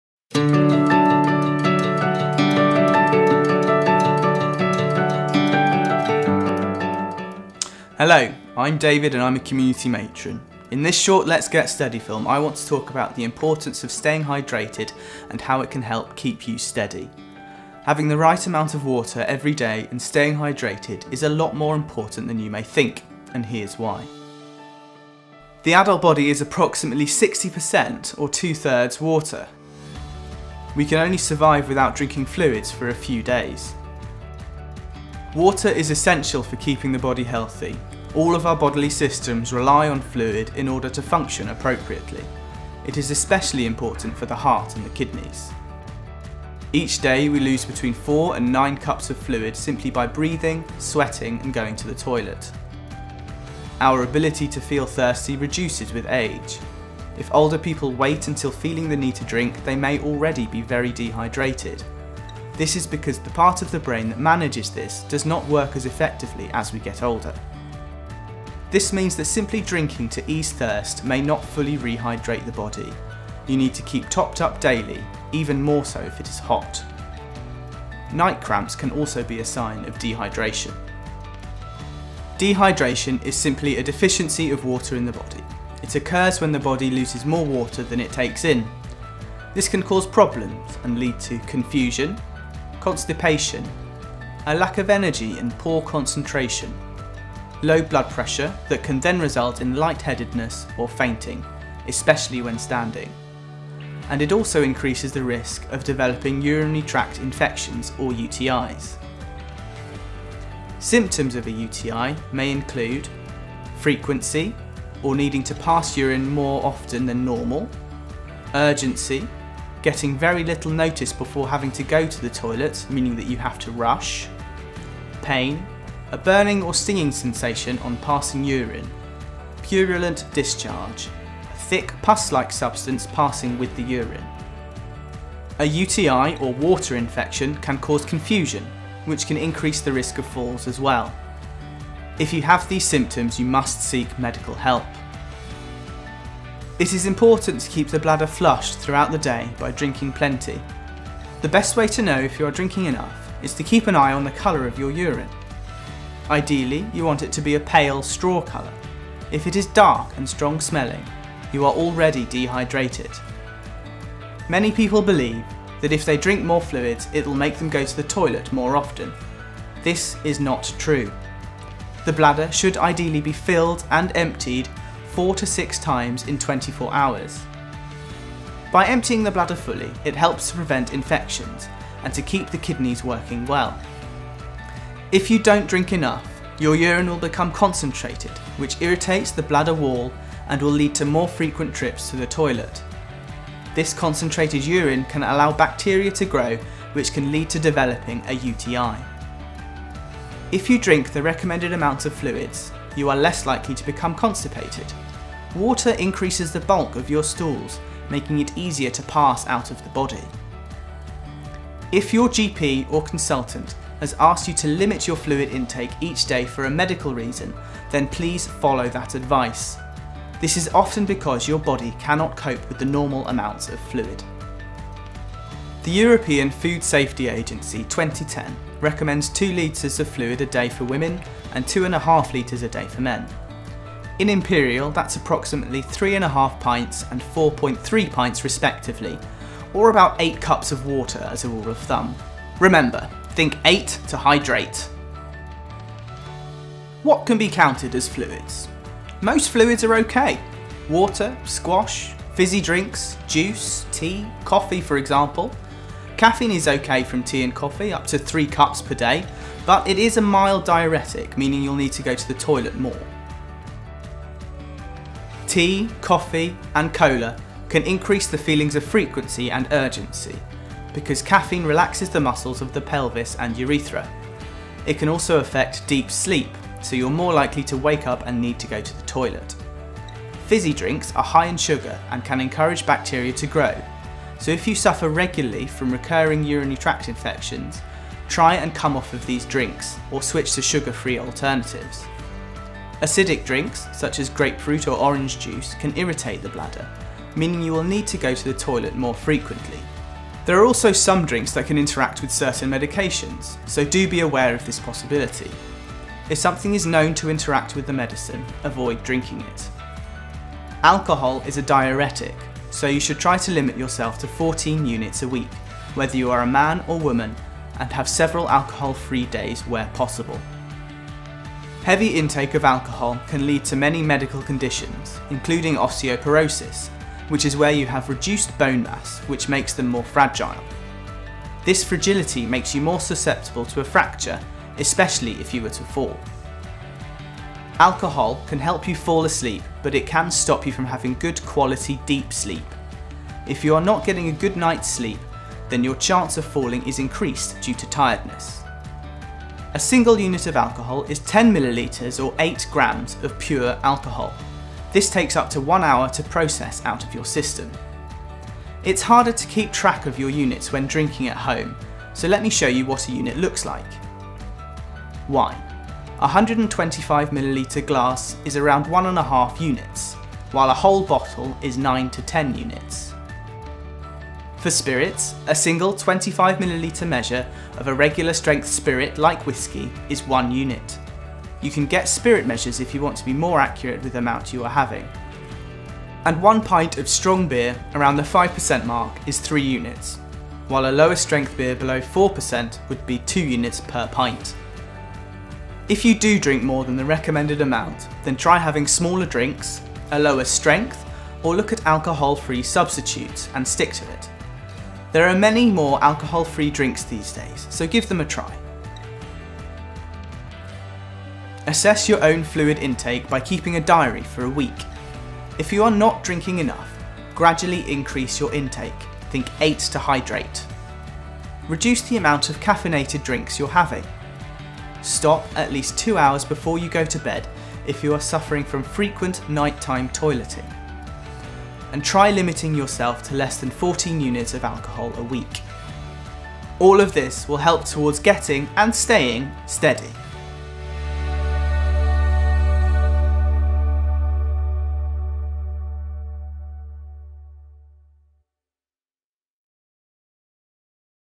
Let's Get Steady Falls Prevention Advice and Information - Hydration
Are you worried about falling? Or have you had a fall that has knocked your confidence, which is now preventing you from doing the things you enjoy?

Let’s Get Steady offers practical advice and guidance to help reduce the risks of falls for individuals who have had a fall, are at risk of falling due to a long-term condition or are worried about falling.

This film will provide you with some key facts around the importance of hydration and how being dehydrated could contribute to a fall.